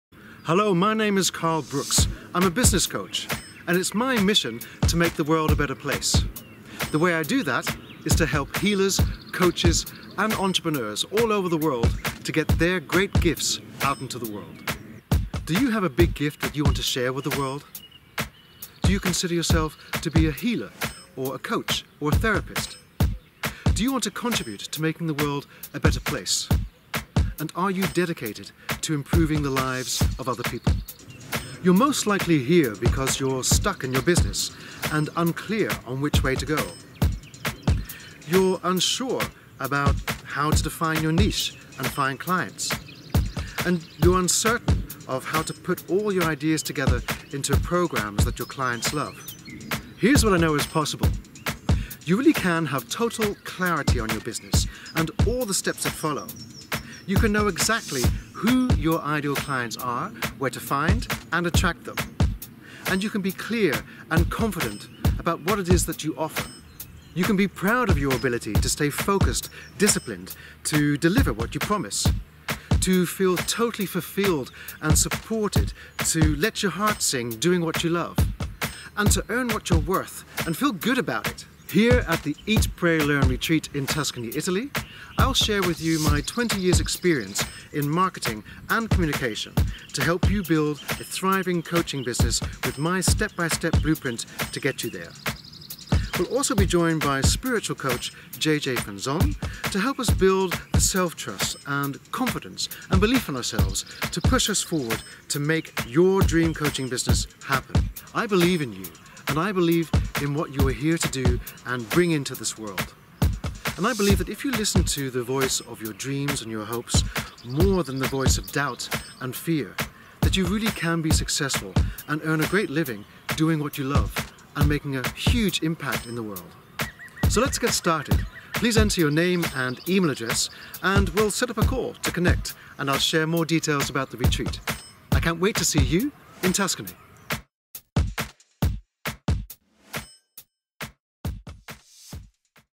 Healers, Coaches and Entrepreneurs Who Want To Make a Difference - Eat Pray Learn Tuscany Retreat
Business Coach Carl Brooks and Spiritual Coach JJ van Zon invite Healers, Coaches, Therapists and other Entrepreneurs who want to make a difference with their dream business to the Eat Pray Learn Tuscany Retreat. You will get the insights on how to create a business and learn how to manage your inner power to keep running it.

Our next retreat is from 1 - 8 October 2016. If you want to make a difference with your dream business, this is for you. Don't miss it. Set up a call.